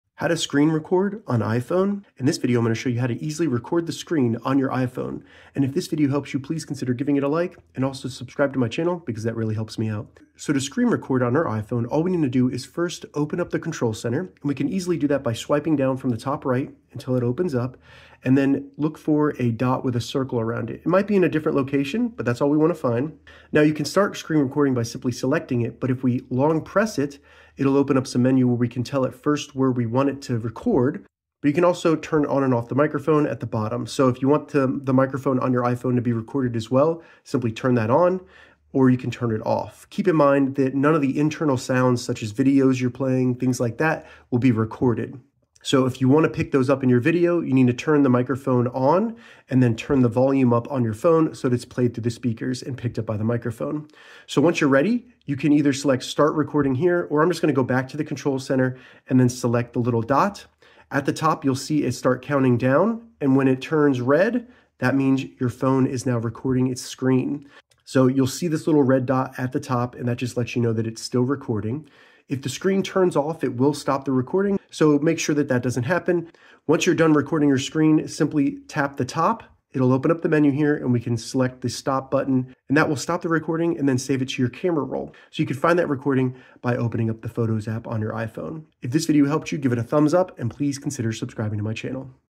How to Screen Record on iPhone - 2025
How to screen record on iPhone? In this tutorial, I show you how to easily record your screen on an iPhone. This means you can screen record games or apps on your iPhone with the built in iOS screen recording tool. Screen recording on iPhone is easy!